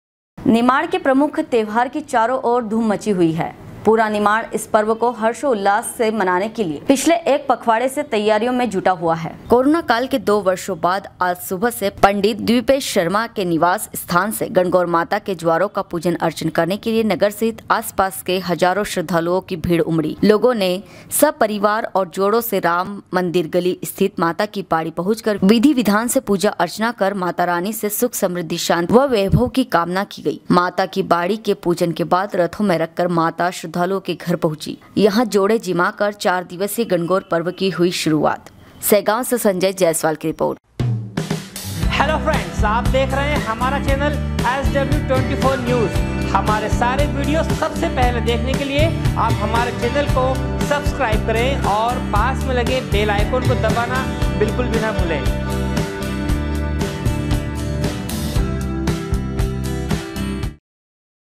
निमाड के प्रमुख त्यौहार की चारों ओर धूम मची हुई है। पूरा निमाड़ इस पर्व को हार्षाेउल्लास
निमाड के प्रमुख त्यौहार की चारों ओर धूम मची हुई है। पूरा निमाड़ इस पर्व को हार्षाेउल्लास उल्लास से मनाने के लिए पिछले एक पखवाड़े से तैयारियां में जुटा हुआ है।